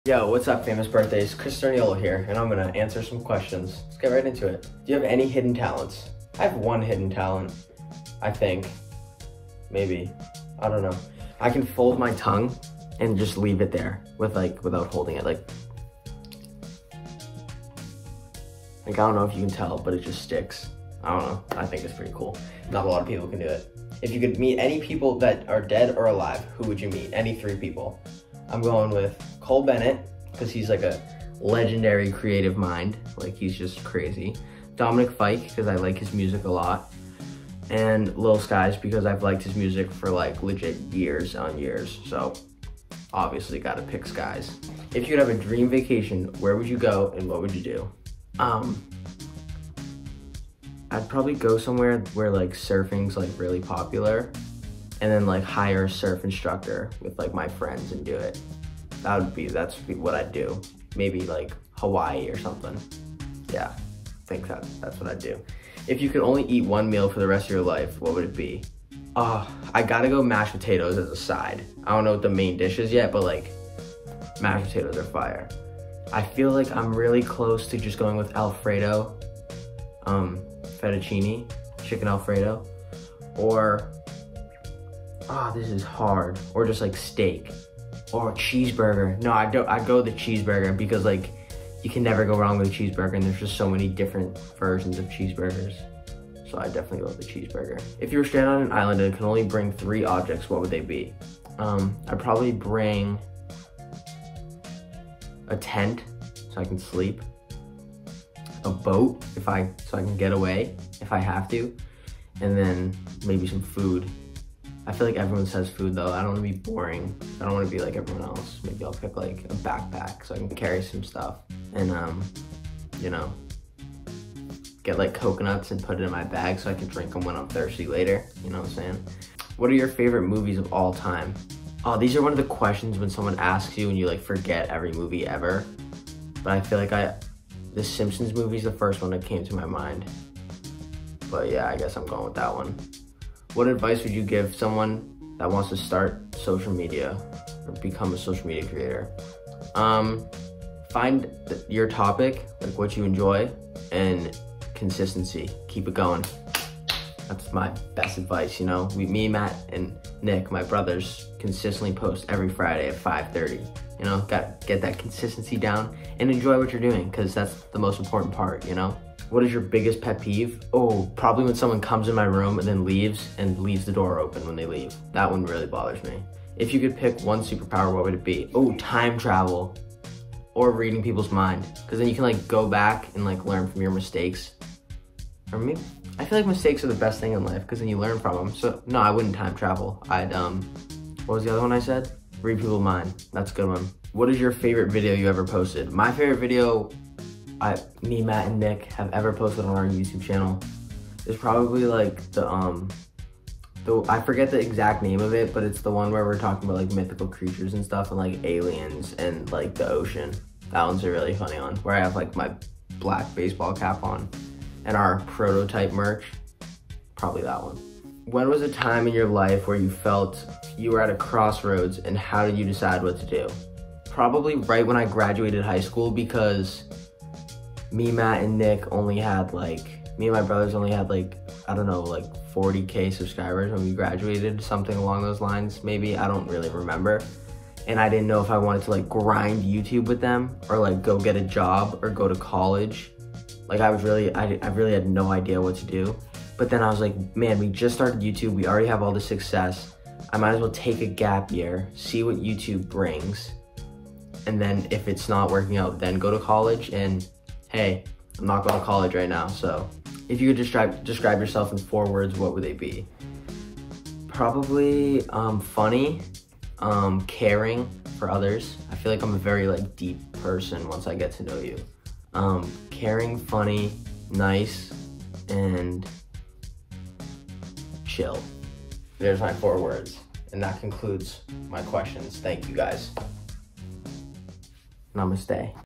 Christopher Sturniolo Q&A
Christopher did a Q&A for his fans on Famous BIrthdays!